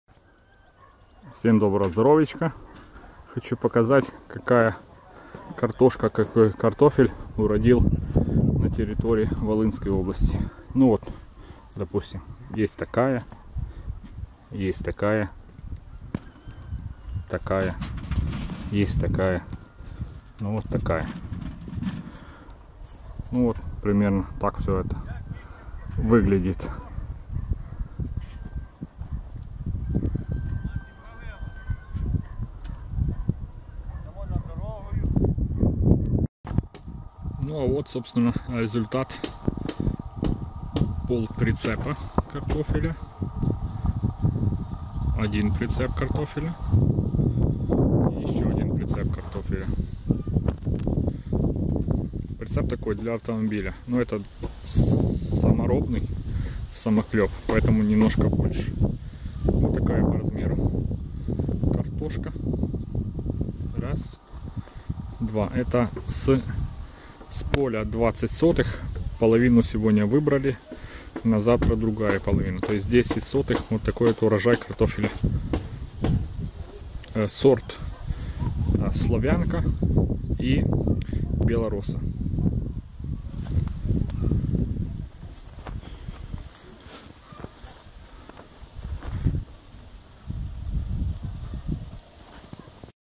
Урожай картофеля - бела роса и славянка
На этих выходных ездили семьей к моим родителям - убирали картофель. За один день полностью собрали клубни с 20 соток поля, урожайность никто ведрами или мешками не измерял, получилась другая мерка - 4 с половиной прицепа. По нашим подсчетам вышло в пределах 3,5 тонны картофеля. Если учитывать засушливое лето 2015 года, то урожай довольно пристойный.